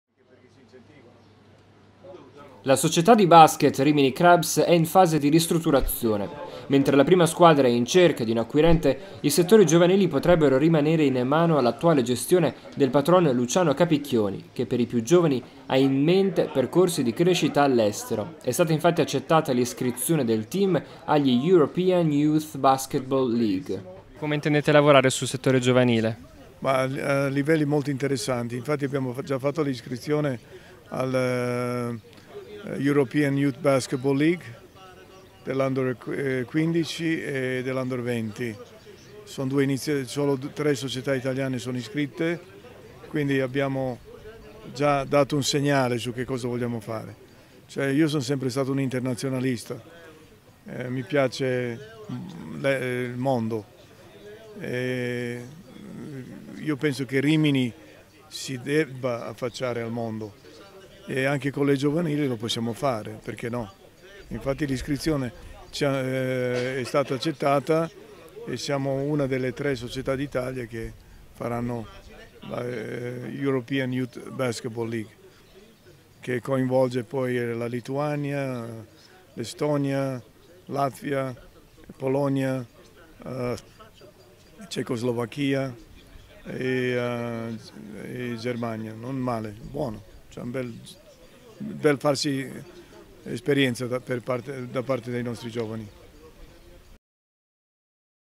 BASKET: Giovanili Crabs Rimini iscritti a European Youth Basketball League | VIDEO
La società di basket Rimini Crabs è in fase di ristrutturazione. Mentre la prima squadra è in cerca di un'acquirente, i settori giovanili potrebbero rimanere in mano all'attuale gestione del patron Luciano Capicchioni, che per i più giovani ha in mente percorsi di crescita all'estero. È stata infatti accettata l'iscrizione del team agli European Youth Basketball League.